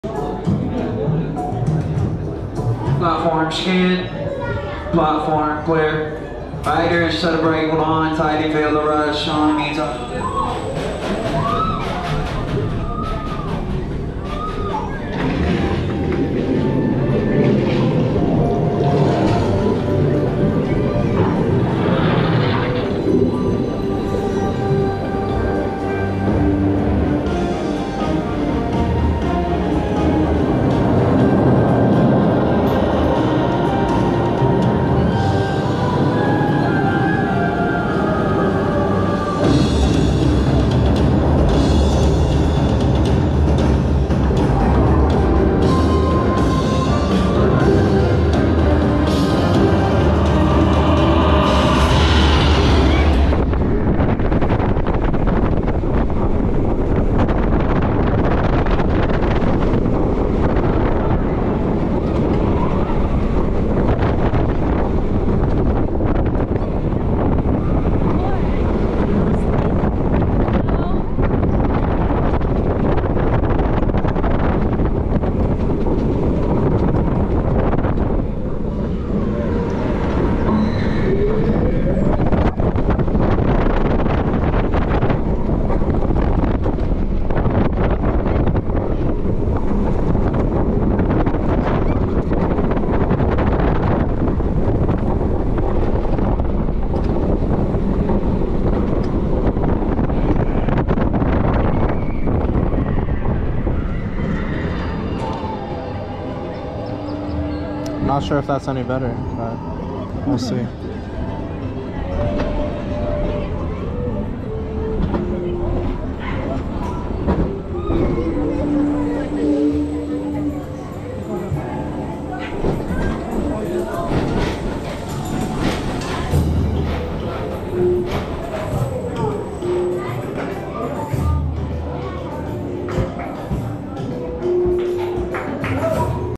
Manta Front Row POV - SeaWorld San Diego
Manta is an Awesome family coaster at SeaWorld!